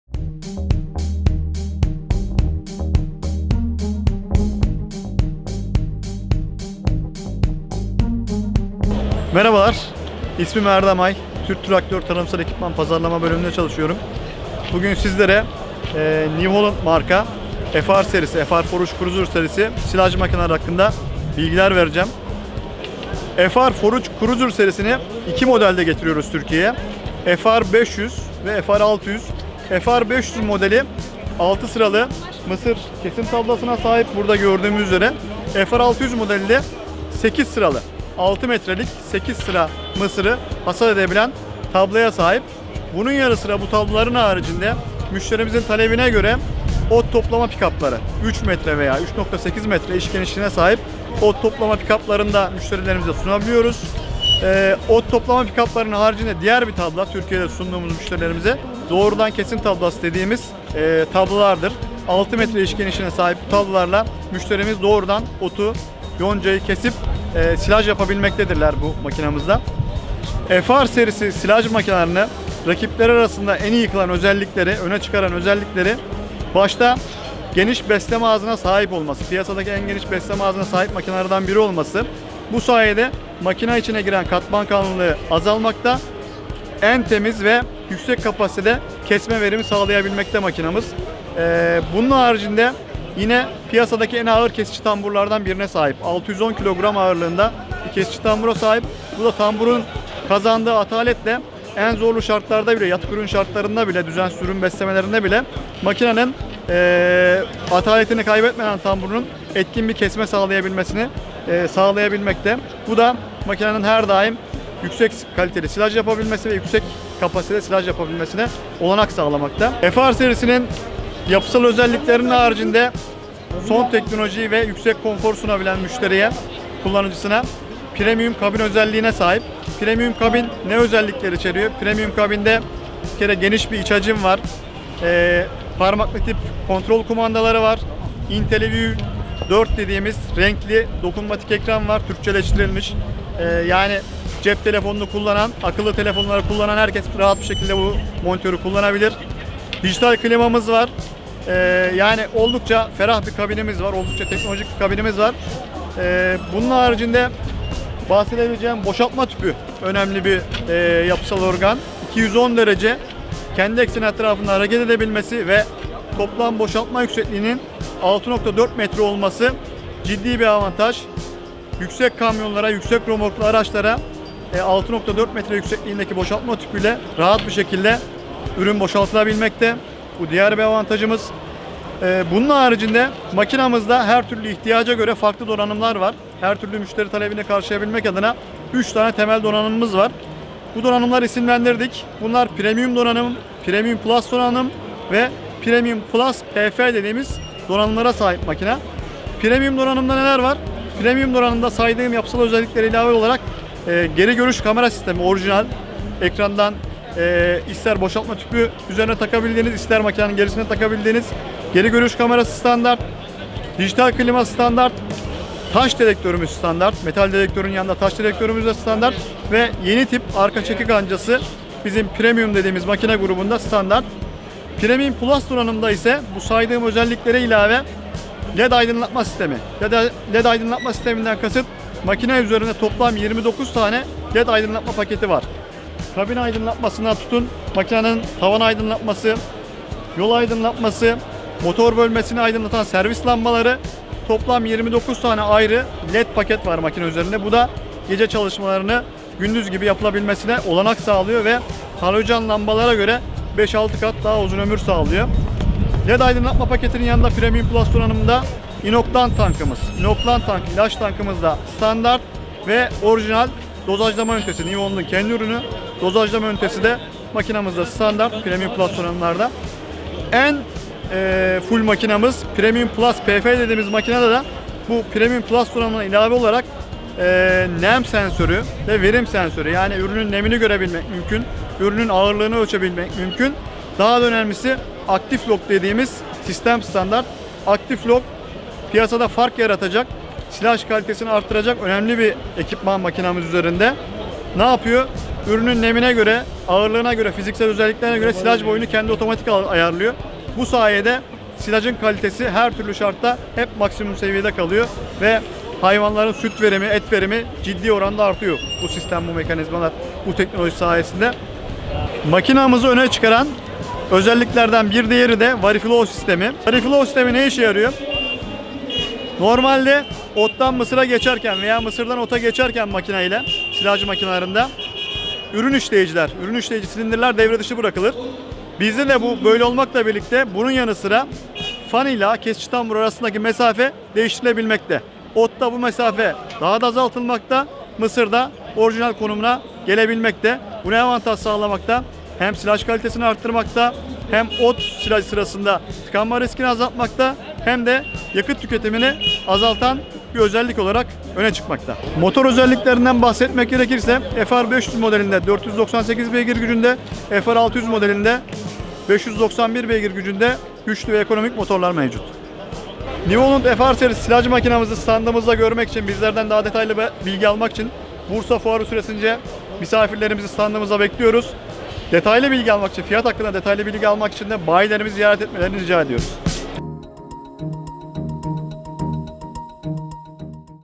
New Holland FR Forage Cruiser Serisi
TürkTraktör Tarımsal Ekipman Pazarlama bölümünde ürün pazarlama uzmanı olarak çalışan Erdem Ay, New Holland marka FR Forage Cruiser Serisi hakkında detaylı bilgi veriyor.